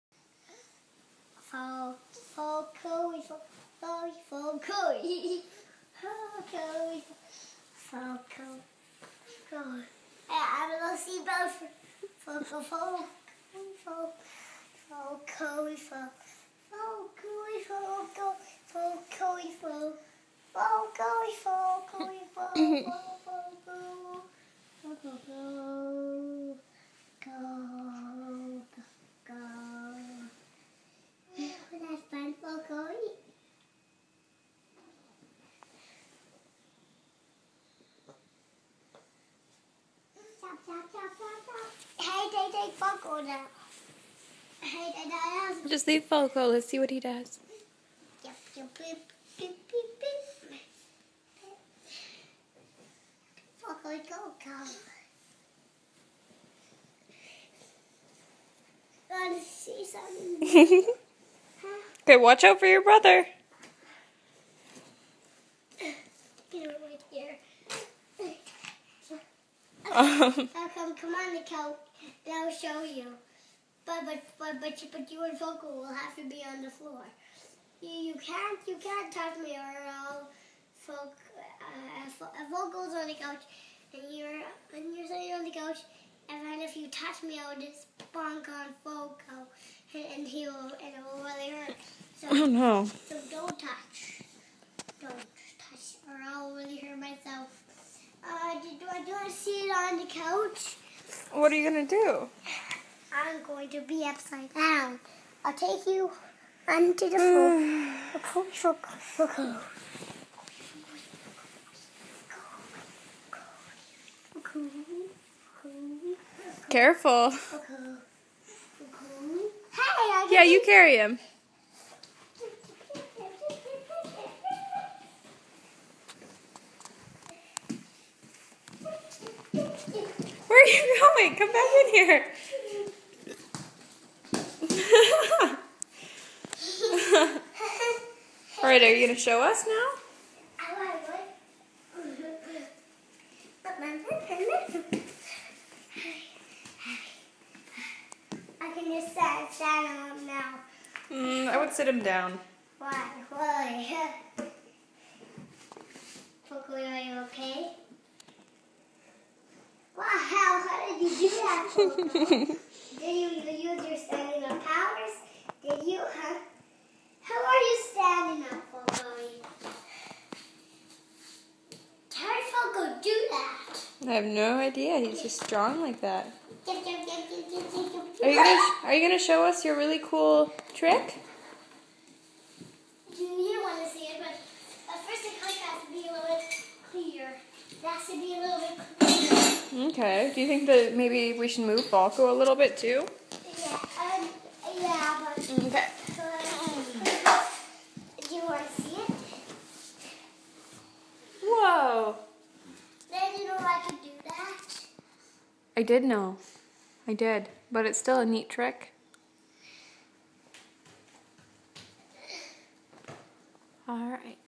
Ramsay, and Falko-y
Ramsay showing off a trick and Falko playing along